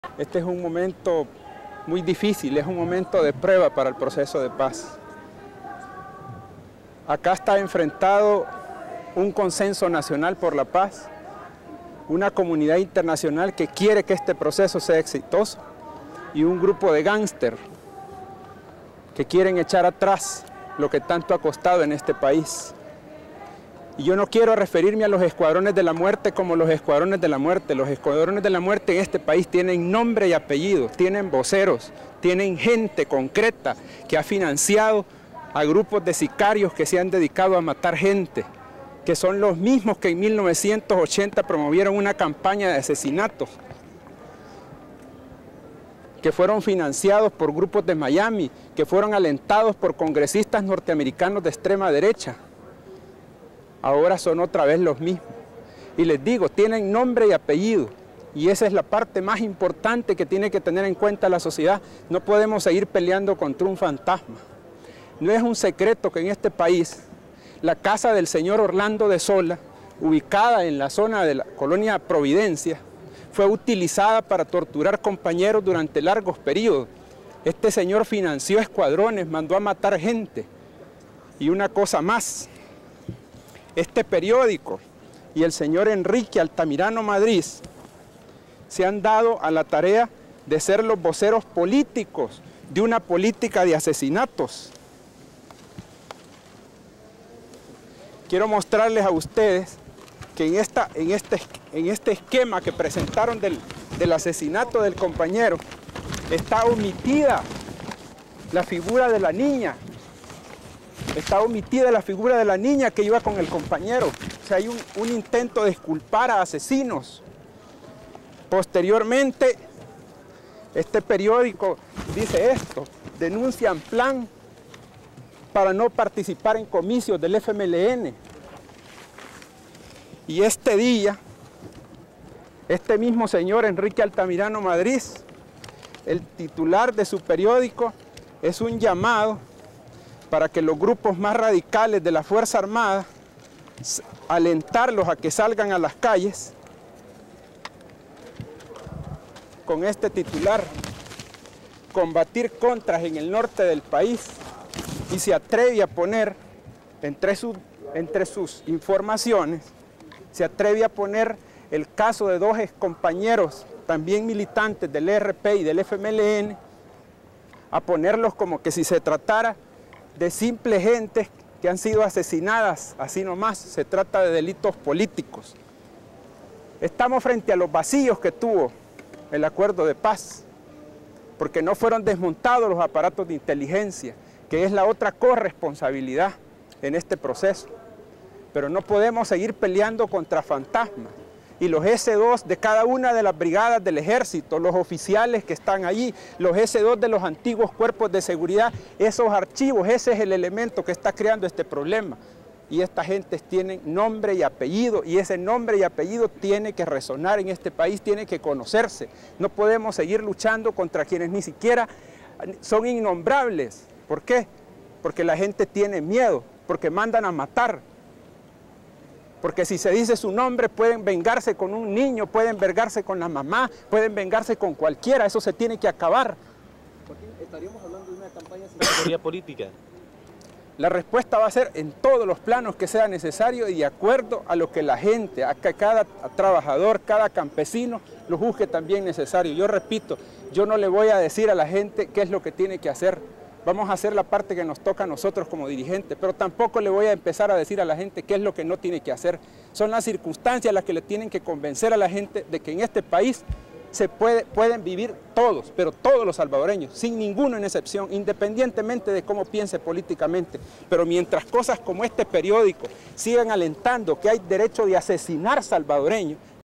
Joaquín Villalobos
Declaraciones realizadas por Joaquín Villalobos en tiempos de conflicto armado.

años80`senElSalvador